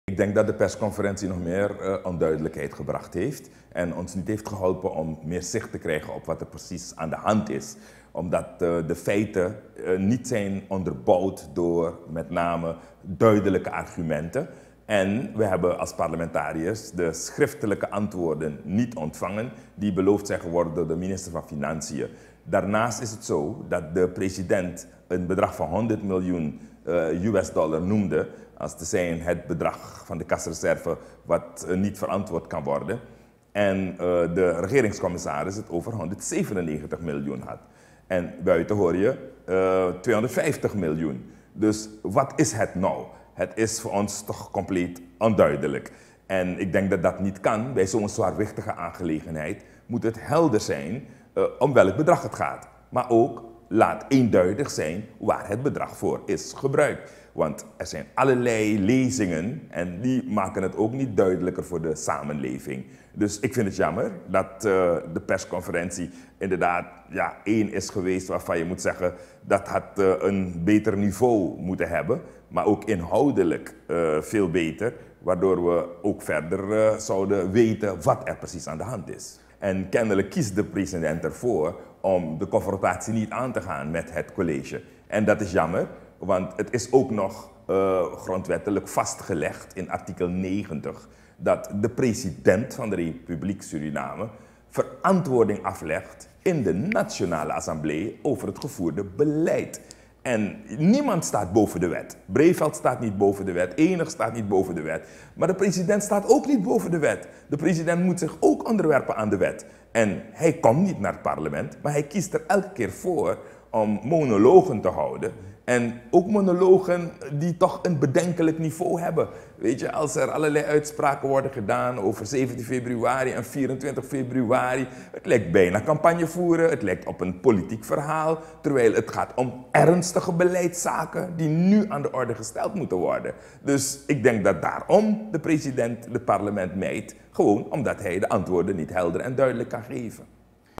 Persconferentie president roept meer vragen op - ABC Online Nieuws
ABC Online Nieuws - 10 februari 2020

Persconferentie president roept meer vragen op

De persconferentie heeft meer vragen opgeworpen meent DOE parlementariër Carl Breeveld.

Door de president, de president van de RVC en door de minister zijn 3 verschillende bedragen genoemd in het kasreserve schandaal.
Ook voor Breeveld is het na de persconferentie meer dan duidelijk waarom Bouterse niet naar het parlement durft te komen.

Volg onze radiouitzending 1x24u LIVE via de link op deze pagina